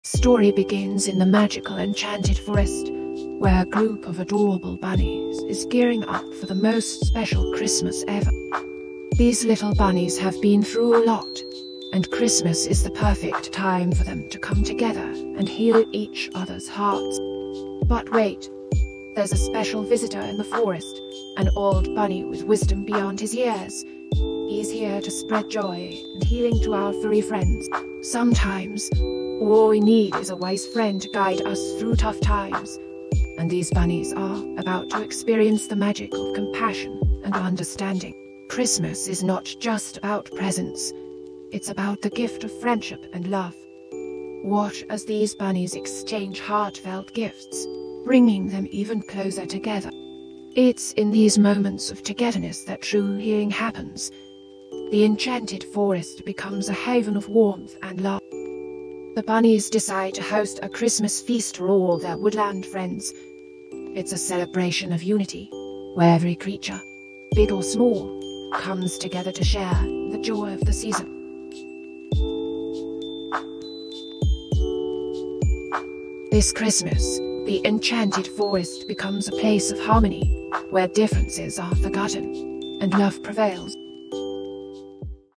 Bunnies Celebrate Christmas (2)
This is how the bunnies celebrate Christmas in the forest. It's about joy and love.